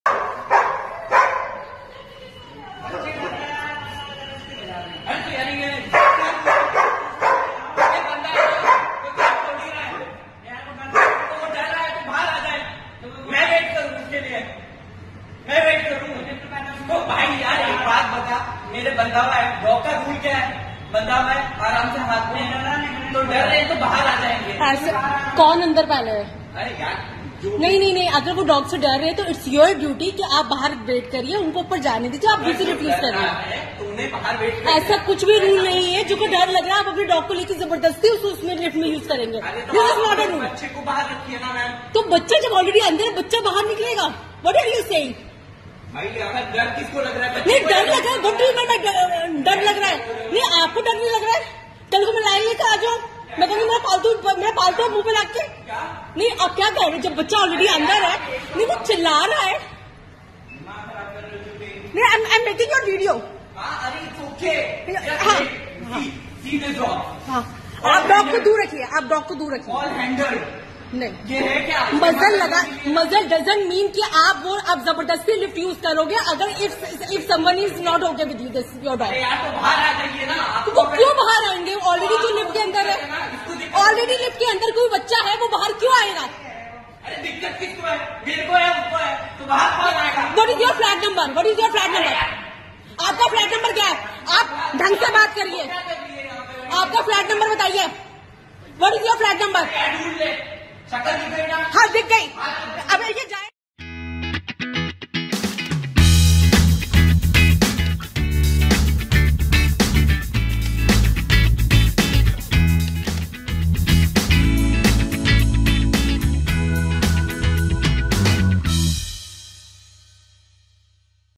लिफ्ट में डॉग ले जाने को लेकर नोएडा में फिर बवाल
ग्रेटर नोएडा के गौर सिटी के सेवेन्थ एवेन्यू में कुत्ते को लिफ्ट में ले जाने को लेकर कहासुनी हुई। दरअसल लिफ्ट में डॉग को देखकर पहले से मौजूद बच्चा रोने लगा। जब महिला और गार्ड ने कुत्ते को लिफ्ट में न ले जाने की बात कही तो मालिक कहासुनी पर उतारू हो गया। महिला ने इस घटना का वीडियो बनाकर सोशल मीडिया पर वायरल किया।